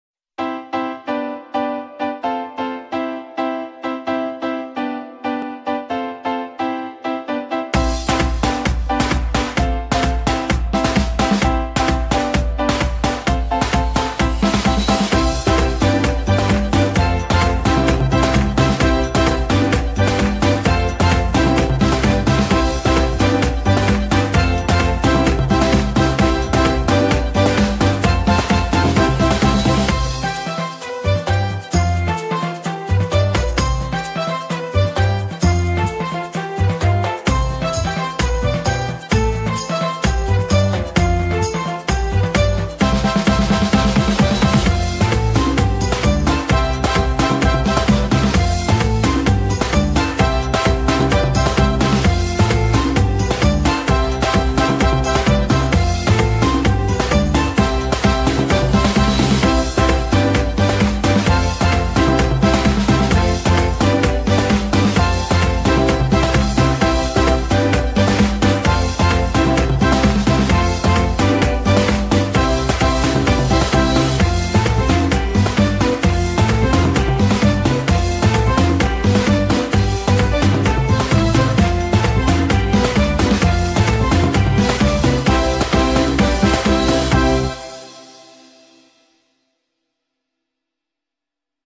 Поздравляю с днем рождения.Универсальная фото заменяемая видео открытка.
Универсальная, фото заменяемая видео открытка с днем рождения для девушки, дочери, внучки,подруги, сестры, любимой.
💖Делитесь с друзьями  видео-открытками., -радуйте их. Здесь всегда найдете новые видео для ПОЗИТИВНОГО НАСТРОЕНИЯ и лучшие поздравления к праздникам.